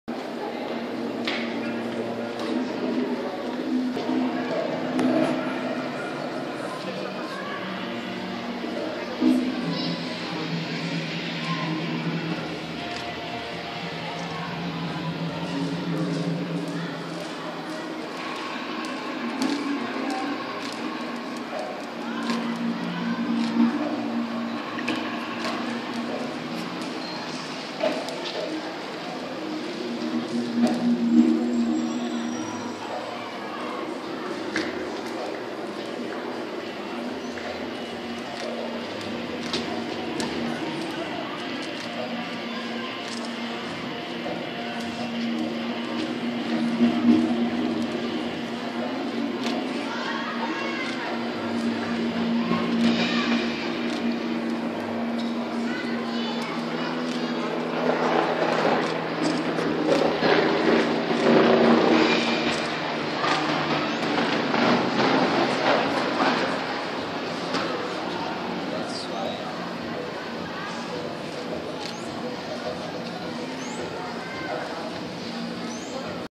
WEB TV - Desfile ECO ERA
Desfile BR ECO ERA no  Arquivo Histórico,
Local: Arquivo Histórico de São Paulo - Pça Cel. Fernando Prestes - Bom Retiro